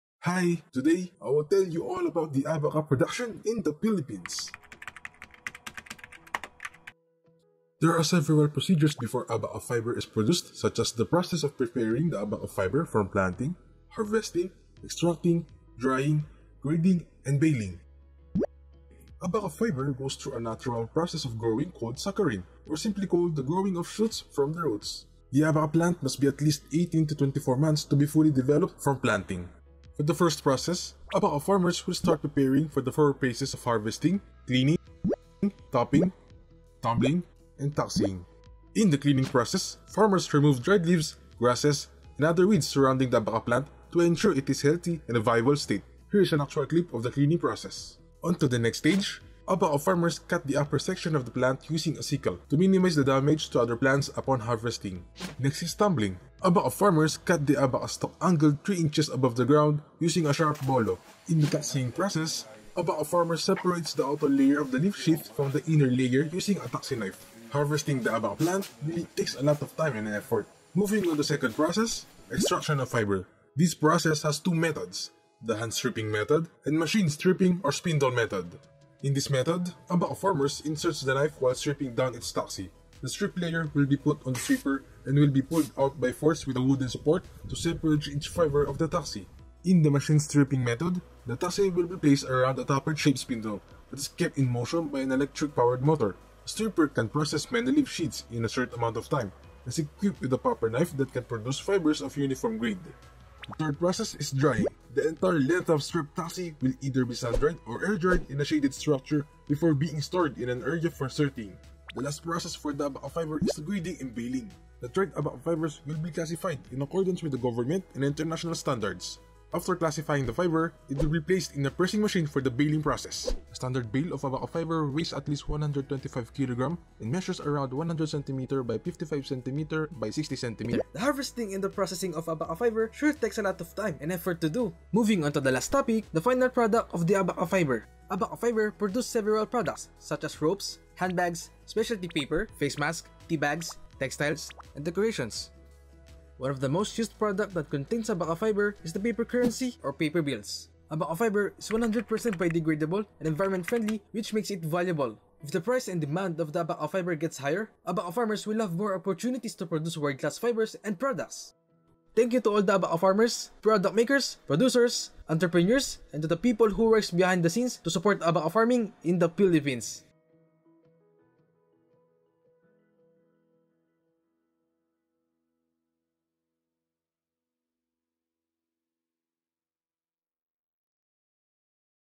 Philippine Abaca Production
It describes the procedures and workflow of the abaca process, from harvesting to the final stages which is grading and baling.